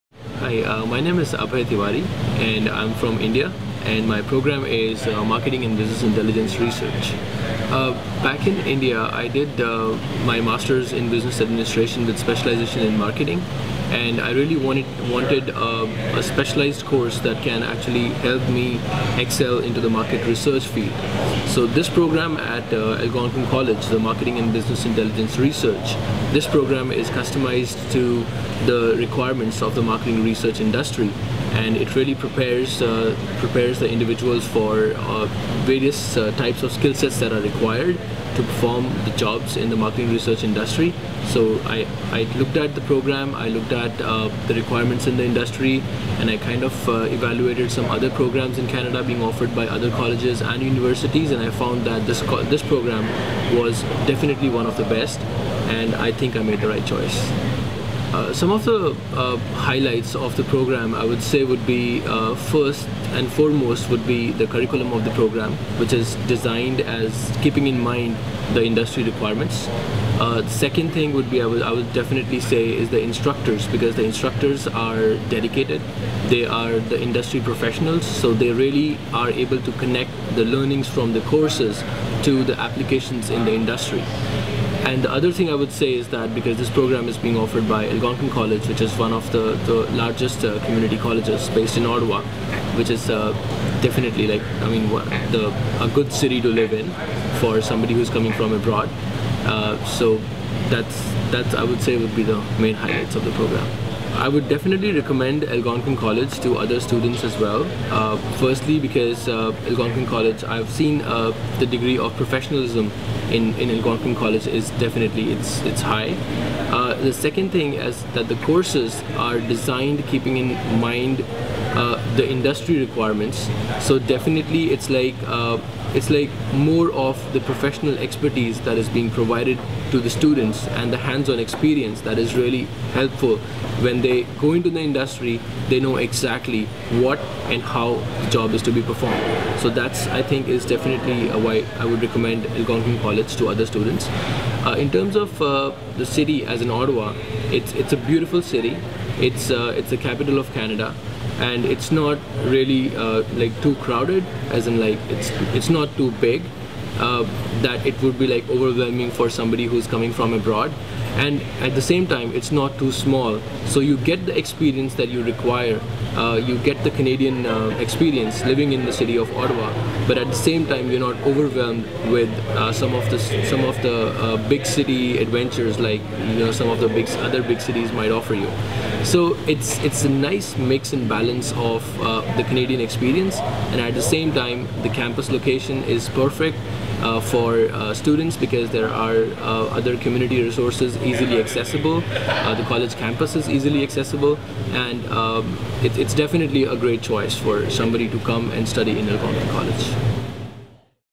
Algonquin College student Abhay Tiwari talks about Marketing and Business Intelligence Research
This Ontario College Graduate Certificate program offered by Algonquin College in Ottawa can launch your career in the dynamic, fast-changing and growing marketing research industry. Students learn how people think, how and why people purchase items and what motivates people to behave the way they do. The program provides students with the employable skills to conduct marketing and opinion research in the public, and private sectors and on the international scene. The curriculum emphasizes the marketing research process including: research design; questionnaire construction; moderator's guide construction; data collection; data base construction; basic and intermediate statistical analysis and interpretation; and report writing skills. In addition, students become familiar with the scientific method of hypotheses testing and reaching conclusions to solve business problems.